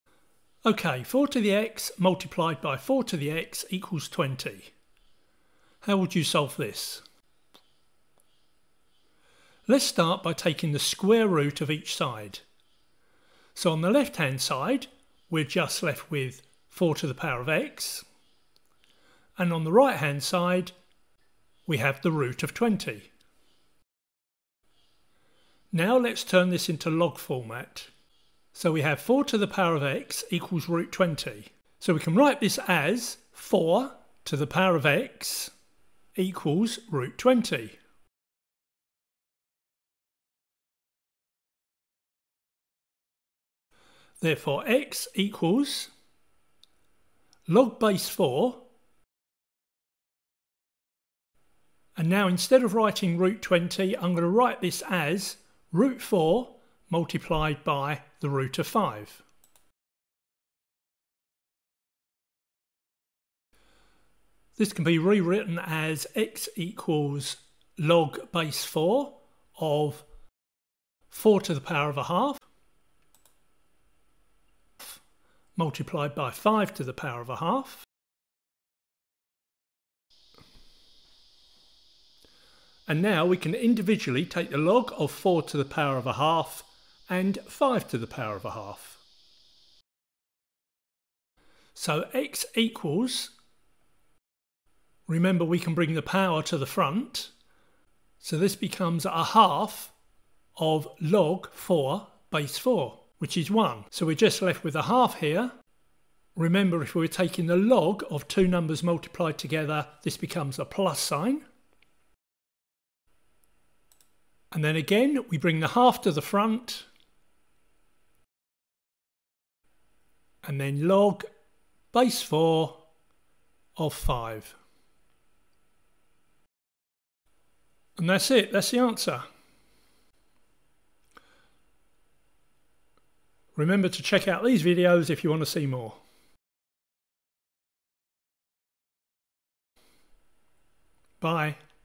Evaluate x the EASY way
How to evaluate x the EASY way - without lots of log manipulation.